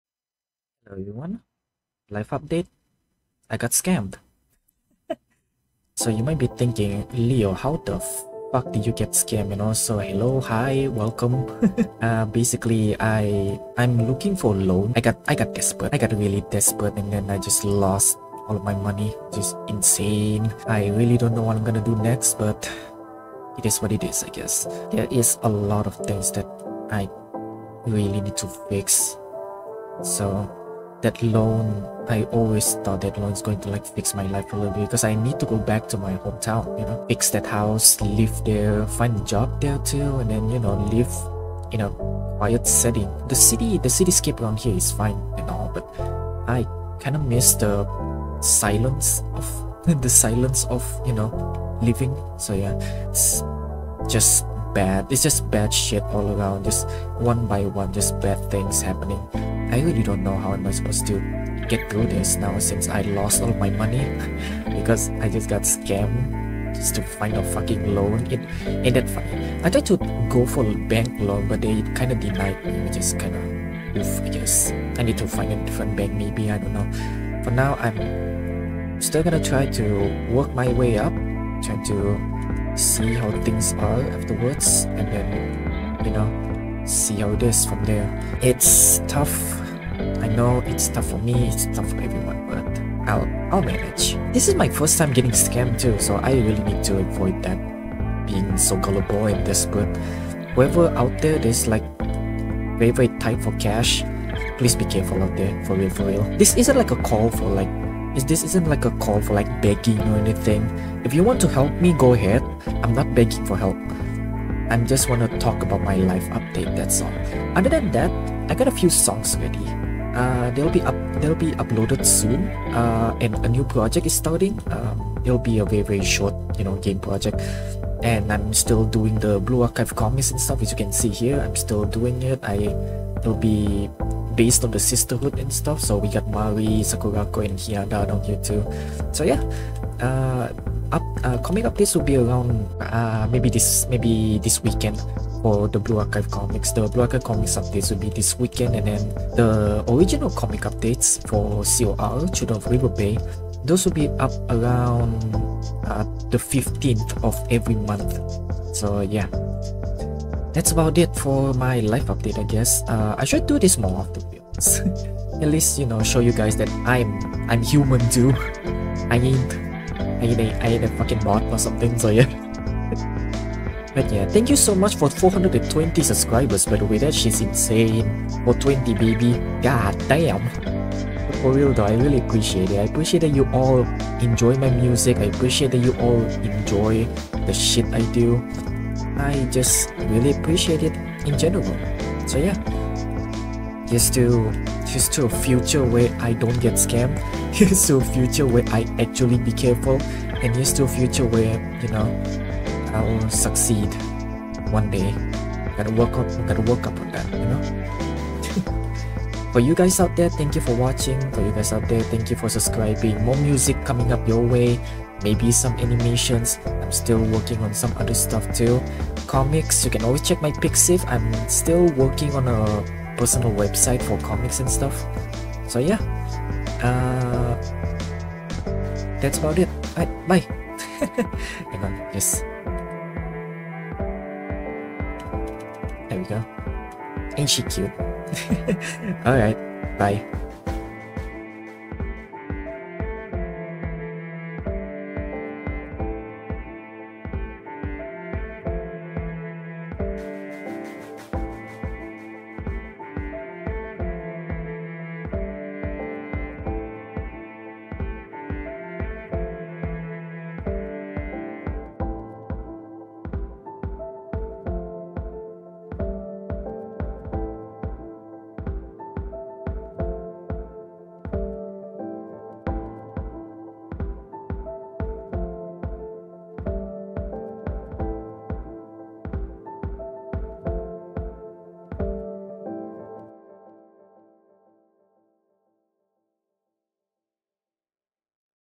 Life Update: I got scammed lol
This video is not a begging video. This is mainly an update since I have other stuff to talk about to.

More stuff coming soon. New music(including this BGM, shameless plugging, I know.) will be up soon.

Be careful out there everyone.
If you get scammed, please, try not to be discouraged, lodge a police report, and just do what you can to take back what's yours from their hands.